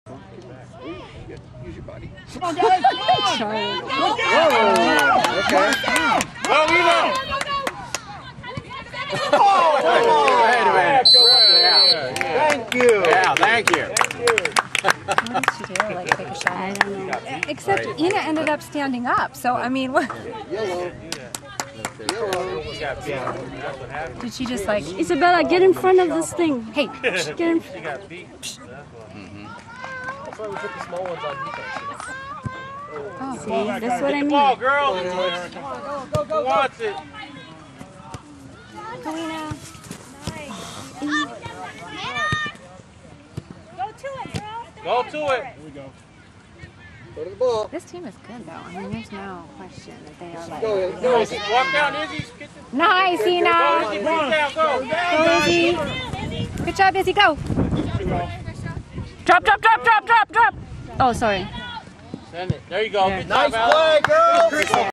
Reichner sisters
Isabella and Inna playing for TM Rage U-12 girls soccer